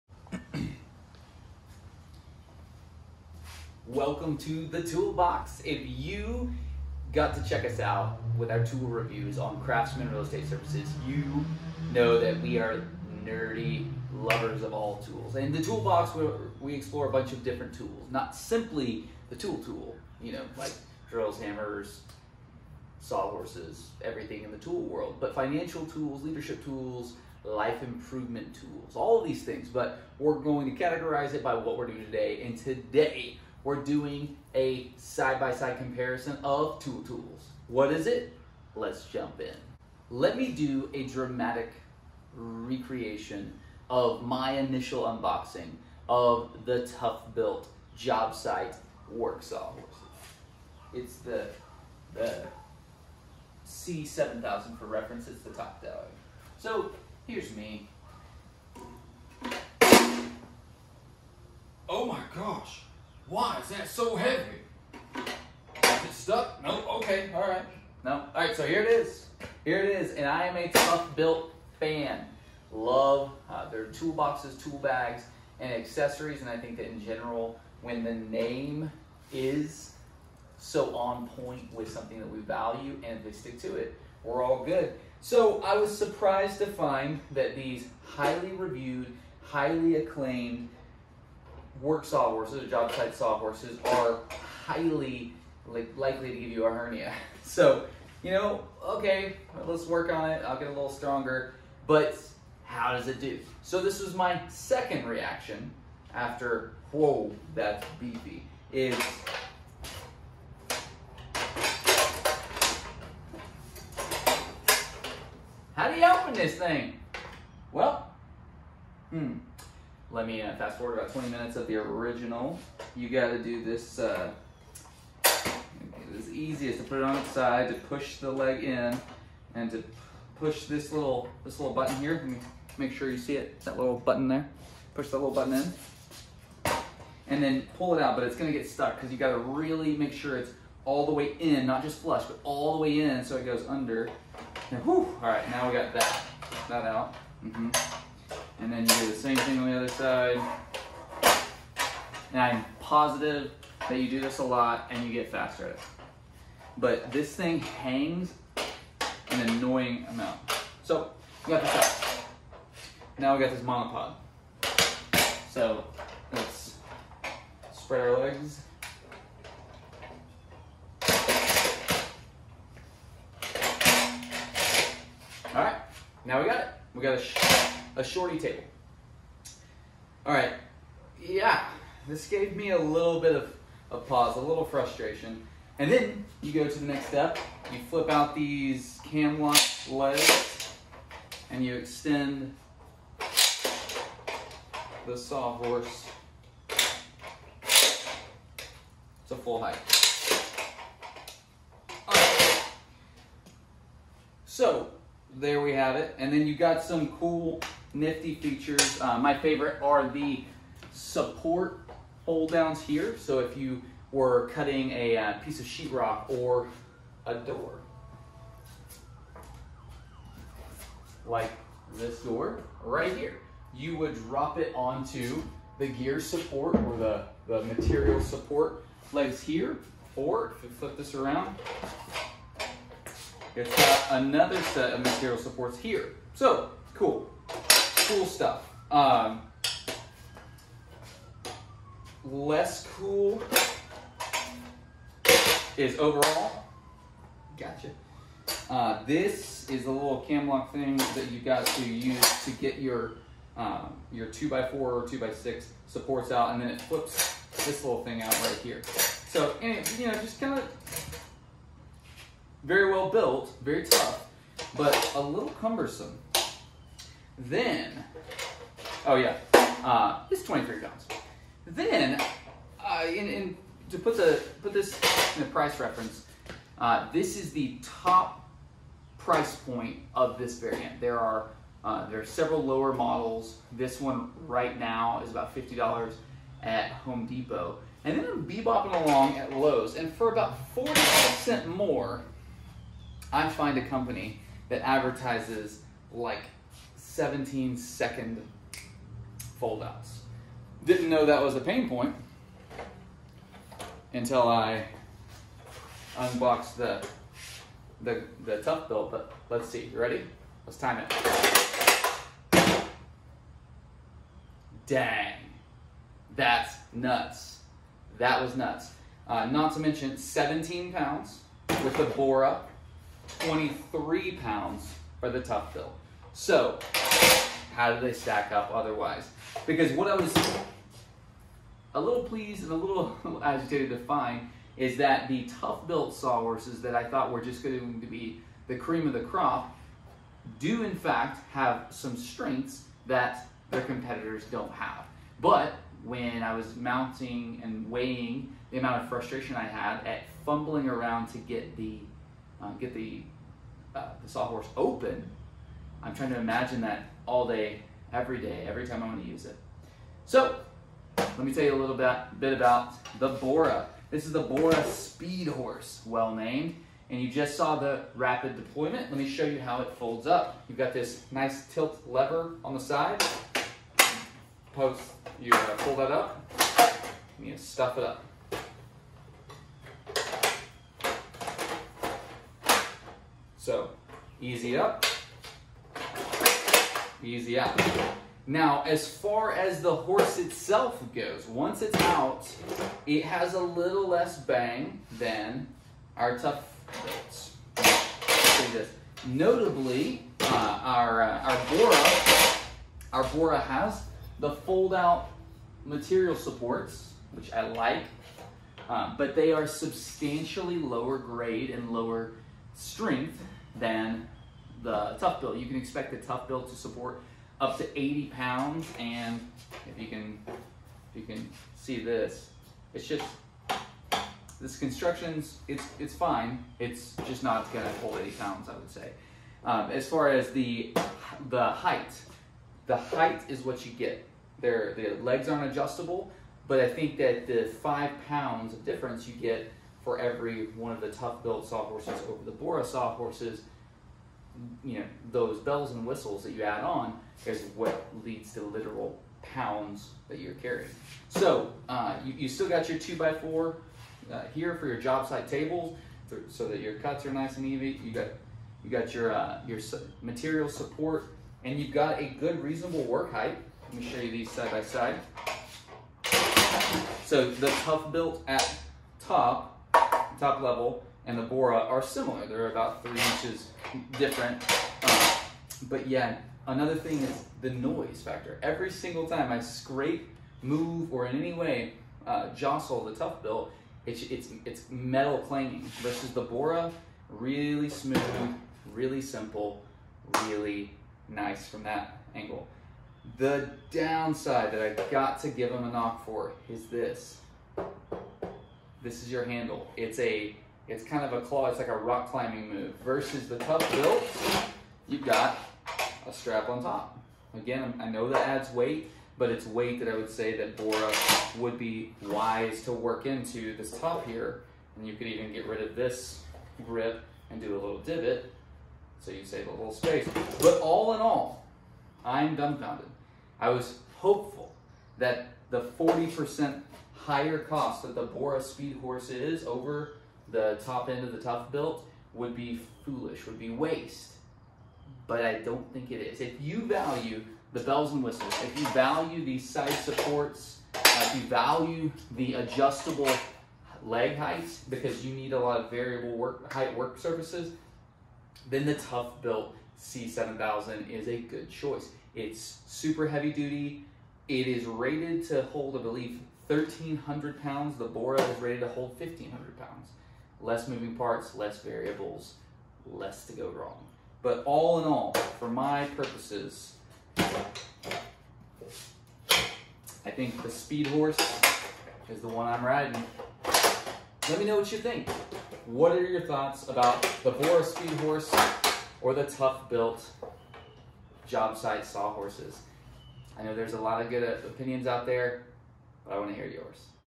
Jobsite sawhorses, Toughbuilt vs Bora - Who is the king?
Let’s dive into these two workhorse job site sawhorses… Both intended and billed as the toughness you need and the best.

Today we will contrast the Toughbuilt C700 and the Bora Speedhorse .

Is one really better than the other? Does one merit a 40% premium over the other? Let’s see . . .